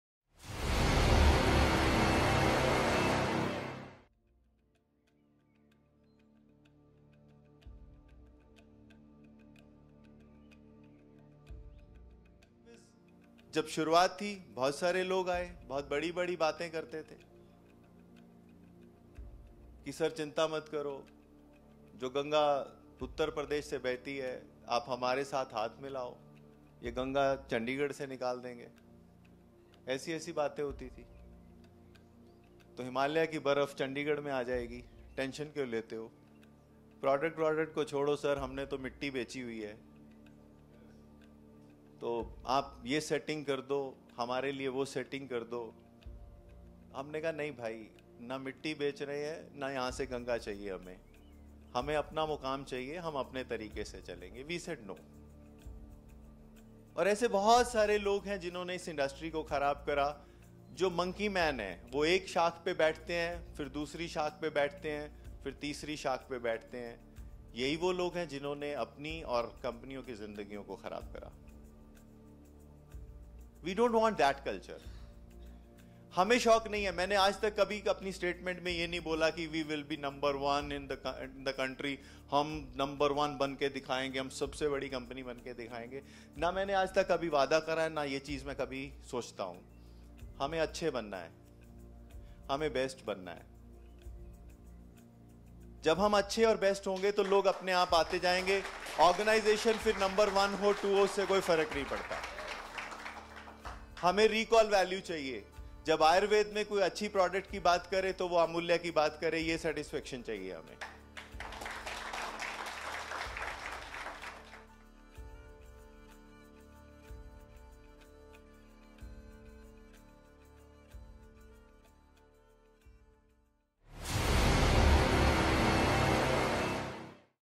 Mr. Manish Marwaha (MD) on a different kind of leaders
Mr. Manish Marwaha (MD, Amulya Herbs Private Limited)